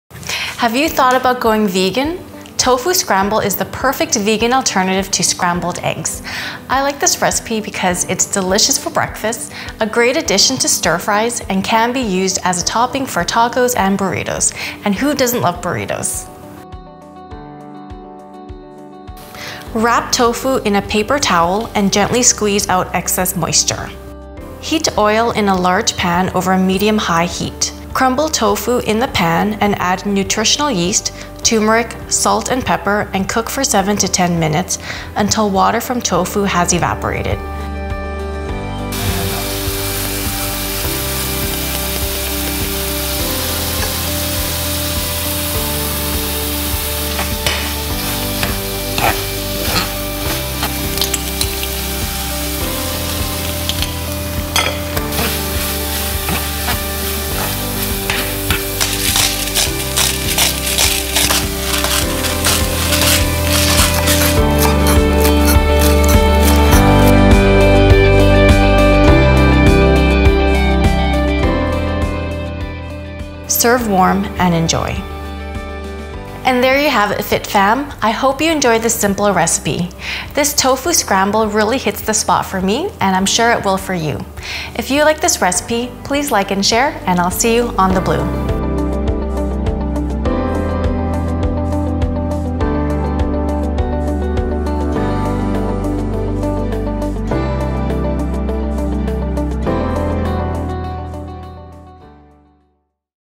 Nary’s Kitchen: Tofu Scramble
Have you thought about going vegan?  Tofu Scramble is the perfect vegan alternative to scrambled eggs!  I like this recipe because it's delicious for breakfasts, a great addition to stir-frys and can be used as a topping for tacos or burritos!

Ingredients:
1 pkg of firm tofu
1 tsp olive oil
¼ cup nutritional yeast
¼ tsp turmeric

Directions:
Wrap tofu in a paper towel and gently squeeze out excess moisture.
Heat oil in a large pan over medium high heat.  Crumble tofu in the pan and add nutritional yeast, turmeric and salt and pepper and cook for 7 - 10 minutes, until water from tofu has evaporated.
Serve warm and enjoy!

And there you have it Fit Fam!  I hope you enjoy this simple recipe!  This tofu scramble really hits the spot for me and I’m sure it will for you!